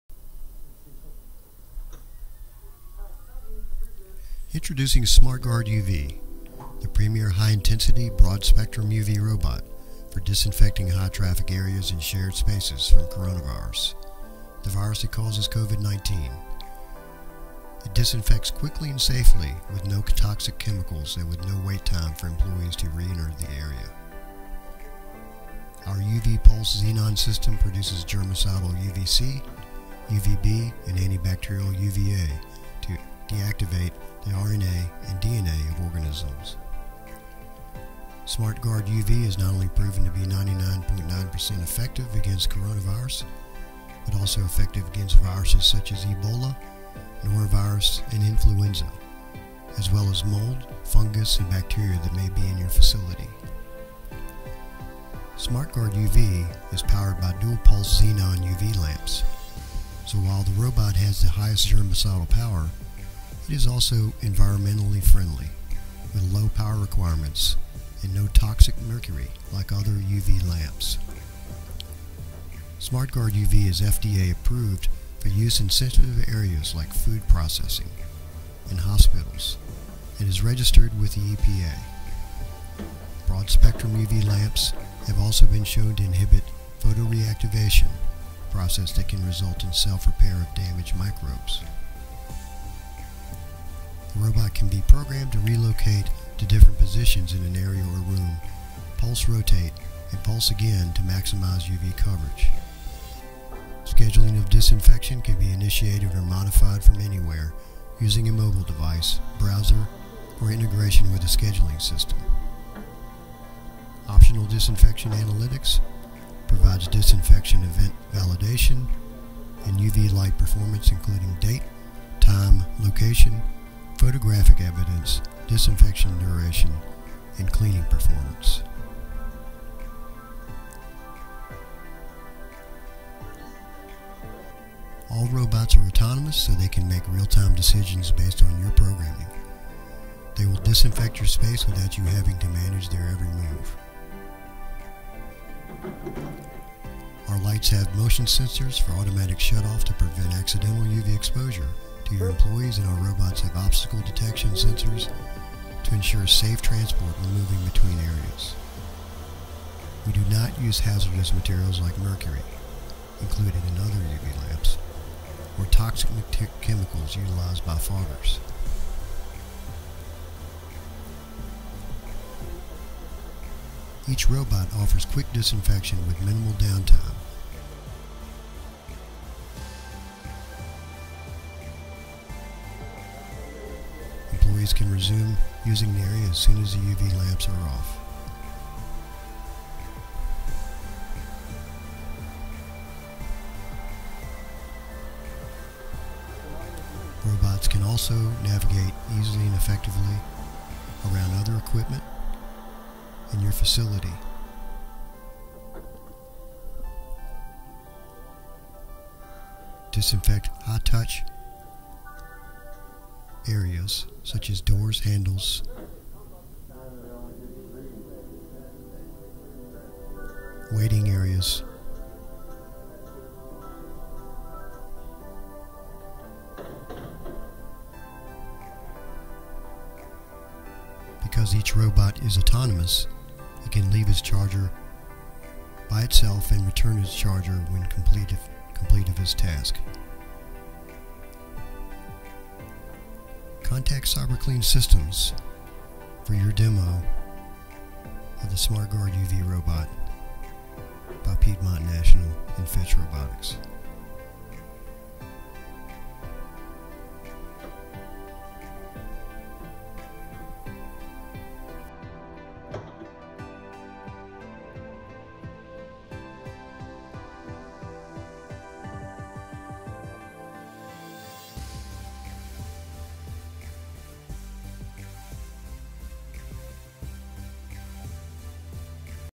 UV Disinfection Robot | SmartguardUV
Cyberclean Systems expert service robot integration team installed  Piedmont National's fully autonomous UV disinfection robot in a Virginia hospital.

The robot needs no human intervention to clean between 250-400 disinfection points on an 8-hour charge.

SmartGuardUV is powered by pulsed xenon UV lamps, so while the robot has tremendous power, it is also environmentally friendly with low power requirements and no chemicals. No toxic mercury.

UV Pulsed Xenon system produces germicidal UV-C, UV-B, and anti-bacterial UV-A to deactivate the DNA and RNA of organisms. PURO* UV Light engines powered by Violet Defense used in the SmartGuardUV are proven to be up to 99.9% effective against bacteria and viruses, including Ebola, Coronavirus, Norovirus, SARS, Salmonella, C.DIFF, E.coli, C.auris, and MRSA.

The robot can be programmed to relocate to different positions in an area or room, pulse, rotate and pulse again to maximize UV coverage. Scheduling of disinfection can be initiated or modified from anywhere using a mobile device, browser, or integration with a scheduling system. Optional disinfection analytics provides disinfection event validation and UV light performance including date, time, location, photographic evidence, disinfection duration, and cleaning performance.